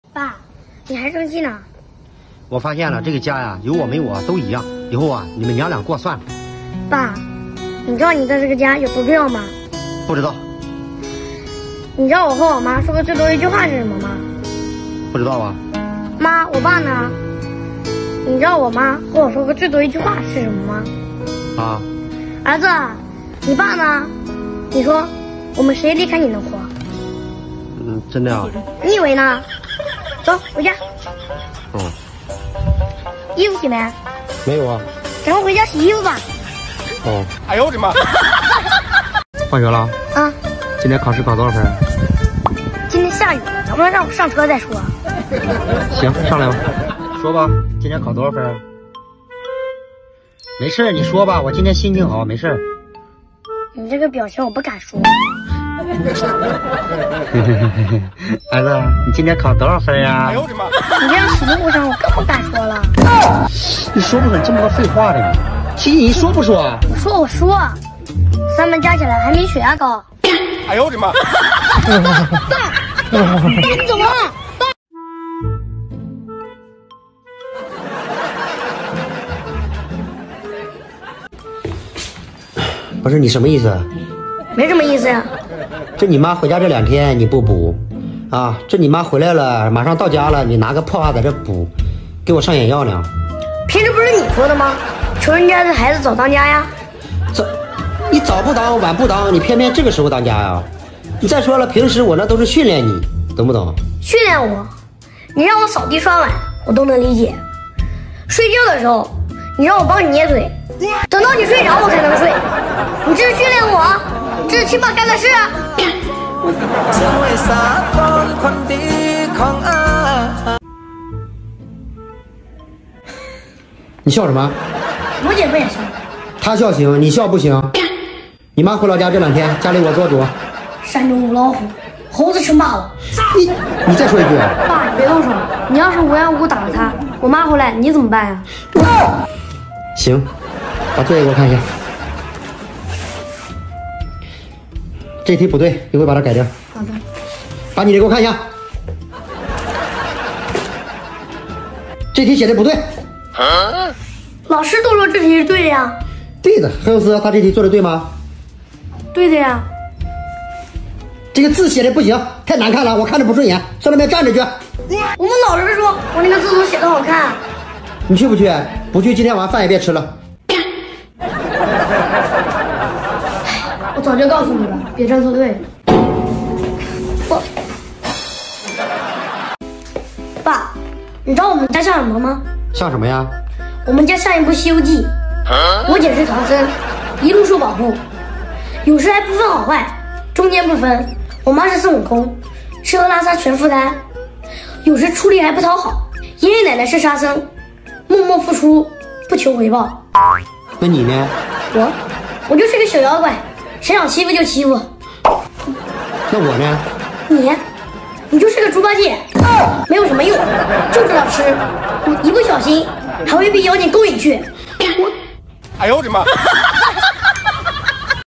老爸生氣離家出走！兒子暖心安慰，這個家缺了誰都不行呀！【快乐父子】
“ 我們製作的影片完全符合平台的使用政策，絕對沒有任何不良內容。” 老爸生氣離家出走！兒子暖心安慰，這個家缺了誰都不行呀！【快乐父子】